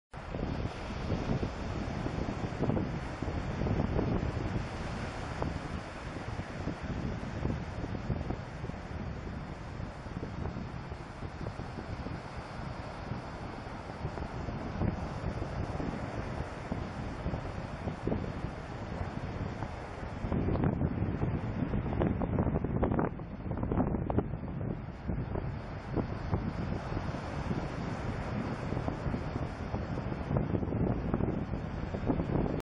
Black-throated Diver, Lunan Bay 10/1/2014
I've checked the Collins book and stills I also took and am confident this bird is a Black-throated Diver but I got it wrong last year. If anyone thinks this is a Red-throated I'd appreciate if you comment here and I'll ask some of my wise club mates to have a look.